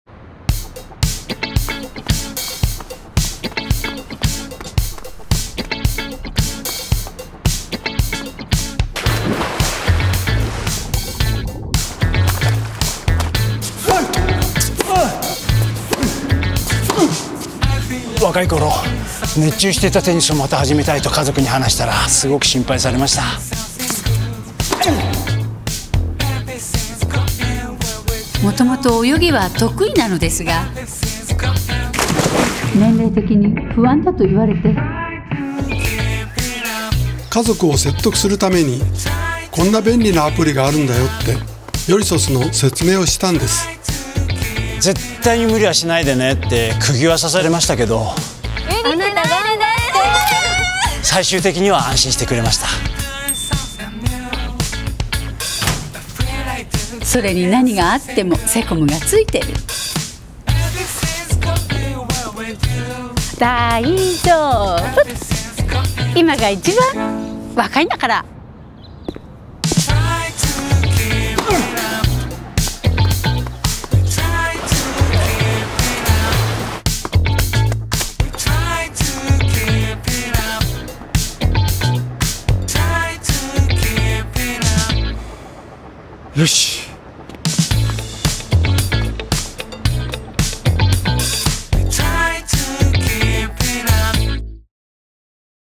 挑むに、寄りそう。「YORiSOS」(コンセプト動画)｜ホームセキュリティのセコム
万が一のときのセコムへの通報サービスと、日々の健康を見守るヘルスケアサービスが融合した、新しいアプリ「YORiSOS（よりそす）」のコンセプト動画です。
「YORiSOS」について詳しくはこちら